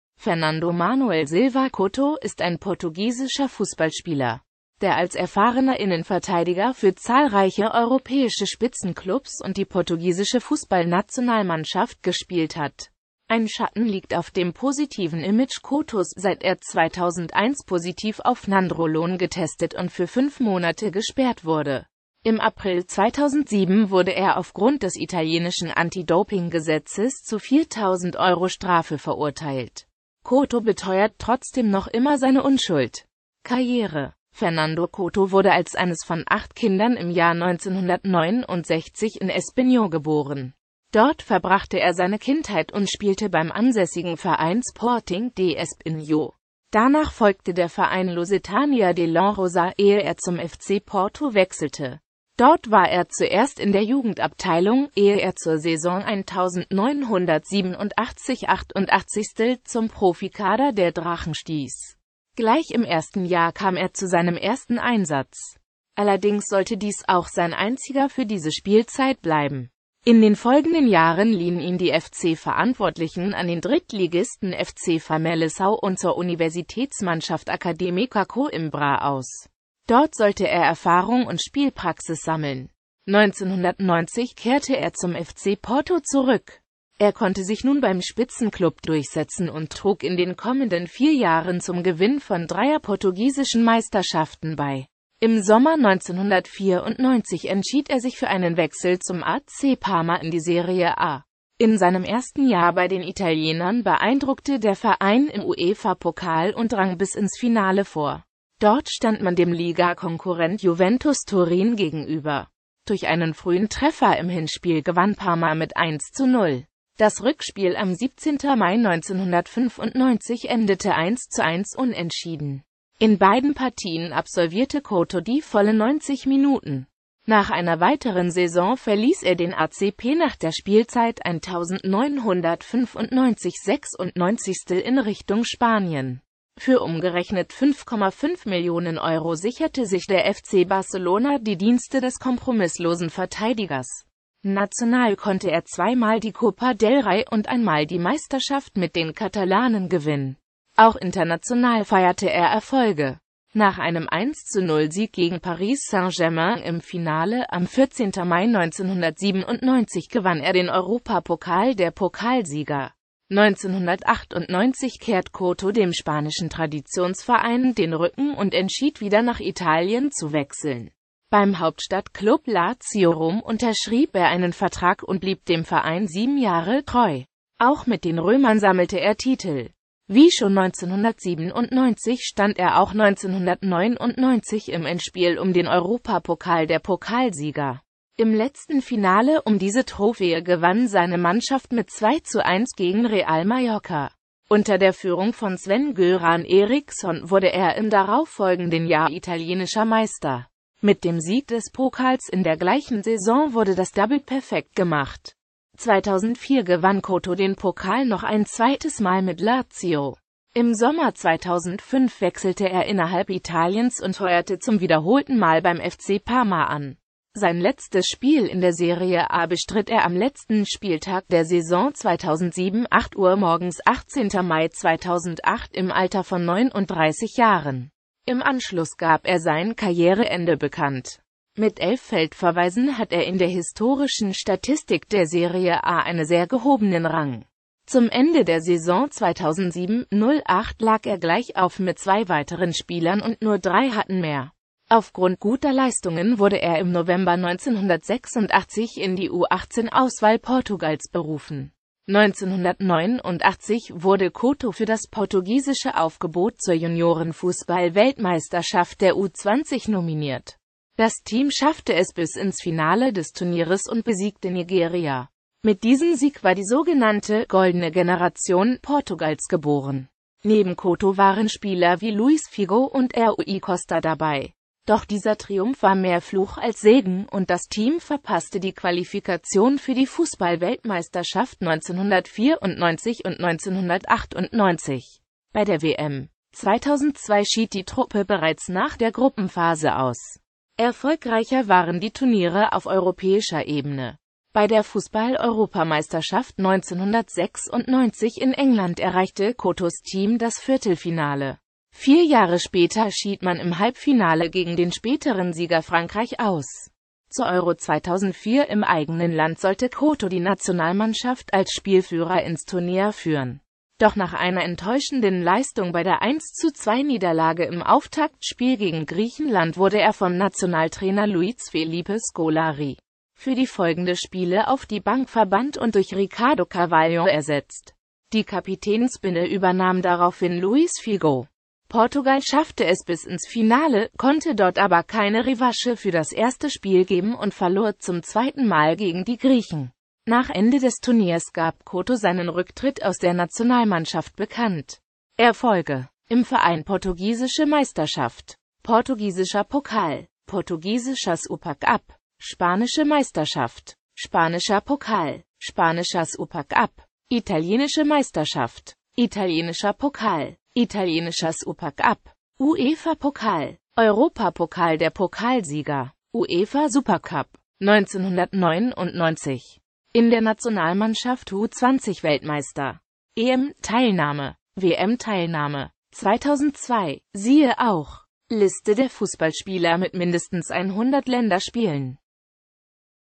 Fernando Couto
Vielen Dank für Ihre Unterstützung:

Fernando Manuel Silva Couto (* 2.August 1969 in Espinho) ist ein portugiesischer Fußballspieler, der als erfahrener Innenverteidiger für zahlreiche europäische Spitzenclubs und die portugiesische Fußballnationalmannschaft gespielt hat.Ein Schatten liegt auf dem positiven Image Coutos, seit er 2001 positiv auf Nandrolon getestet und für fünf Monate gesperrt wurde.Im April 2007 wurde er aufgrund des italienischen Anti-Doping-Gesetzes zu 4000 Euro Strafe verurteilt.